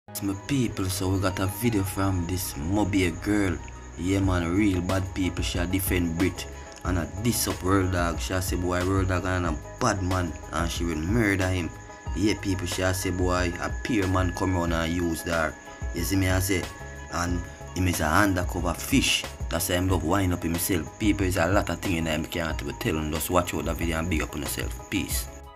MOBAY Choppa Badgyal D3FENDS Brittany & Put World Dawg in his Place
mobay chopppa gyol address world dawg